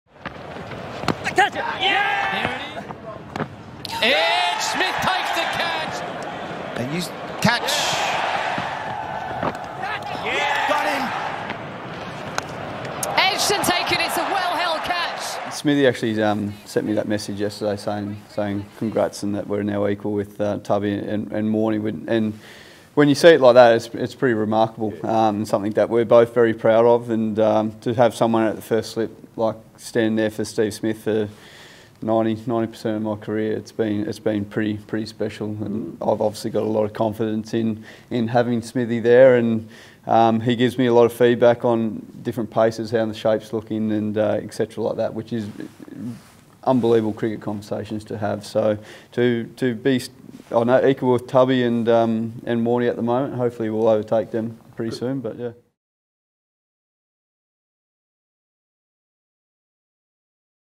Caught Smith, bowled Lyon: Pair equal Australian record | Alinta News Wrap
Alinta News Wrap: Against the West Indies in Perth, the bowler-fielder combination of Nathan Lyon and Steve reached 51 Test dismissals, equalling the record held by Australian cricket legends Shane Warne and Mark Taylor